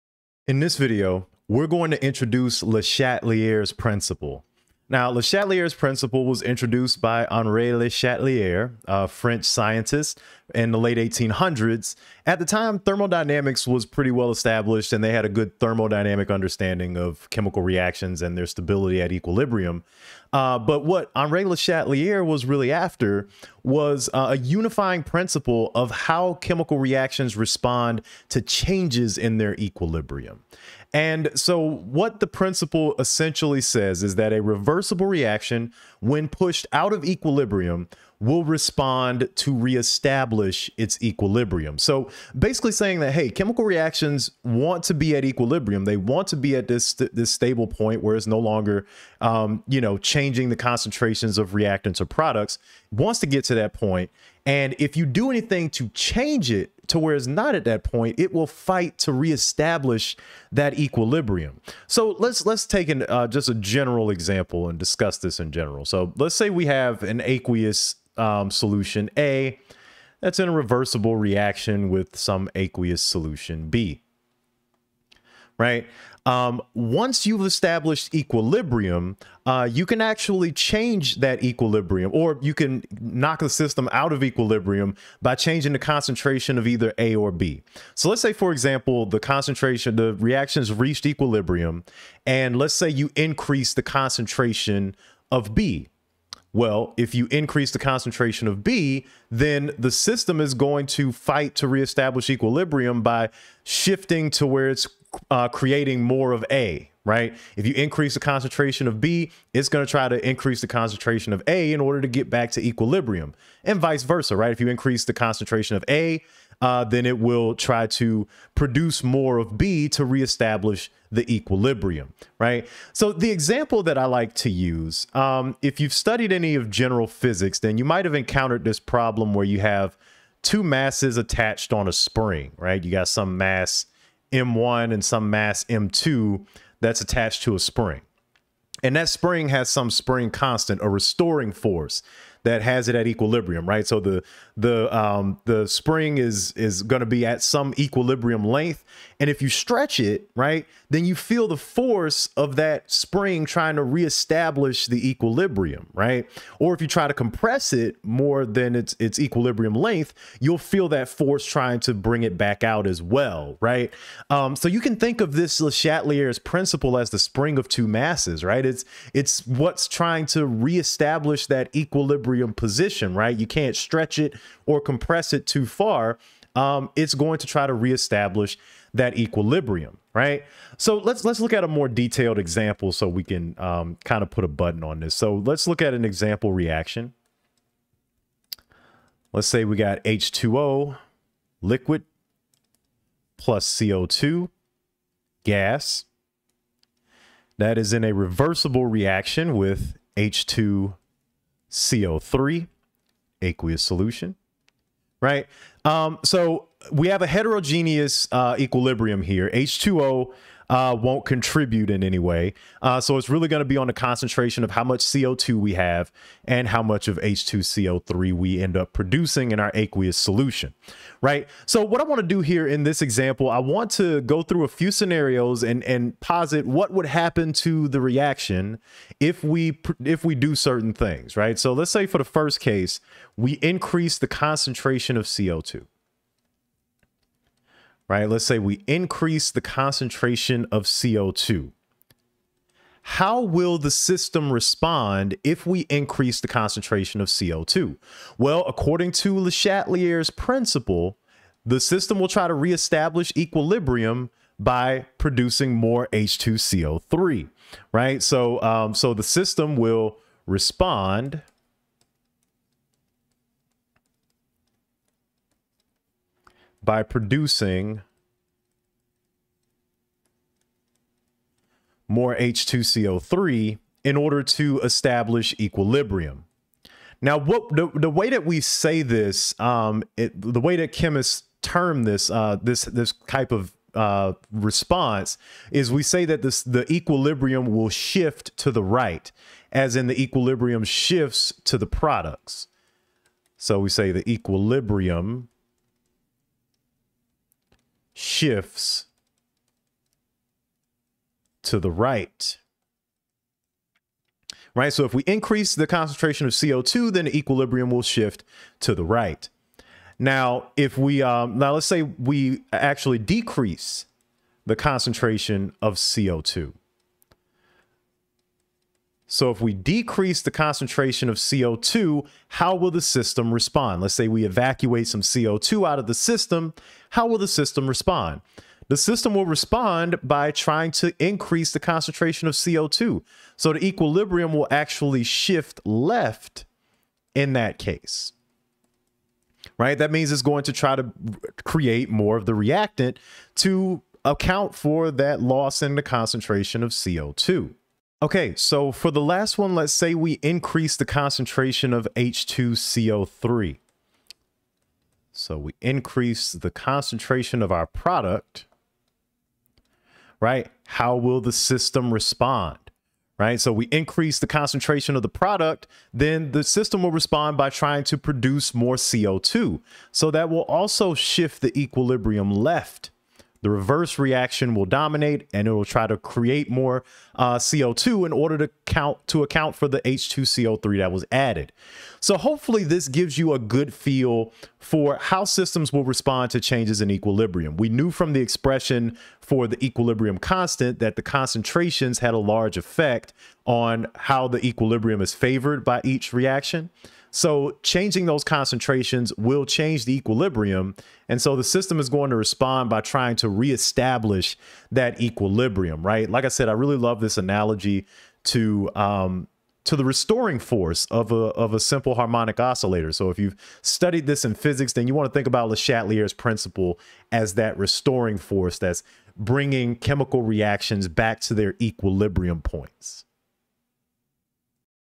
Le Chatelier's Principle | General Chemistry II | 7.5
Chemistry lecture introducing Le Chatelier's principle, this principle was established in order to provide a general theory for how chemical systems respond to equilibrium.